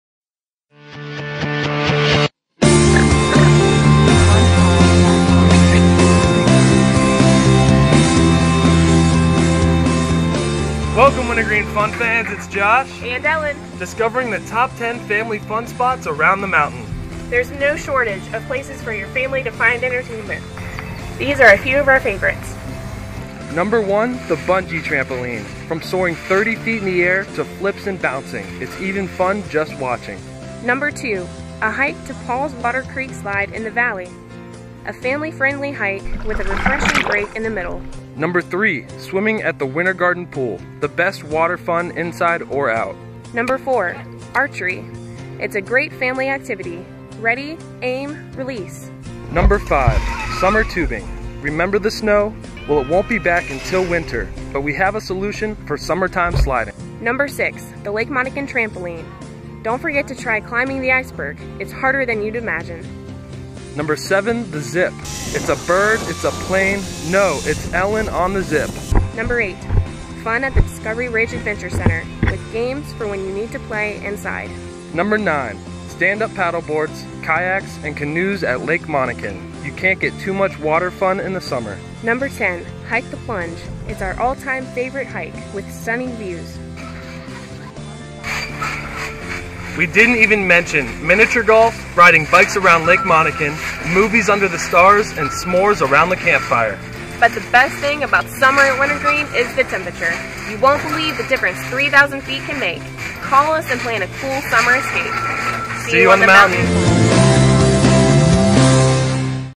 Mountain Minute - Top 10 Family Fun Spots!
From Discovery Ridge to Lake Monocan and everything in between, Josh and Ellen will let you in on all the best spots for family fun at Wintergreen Resort!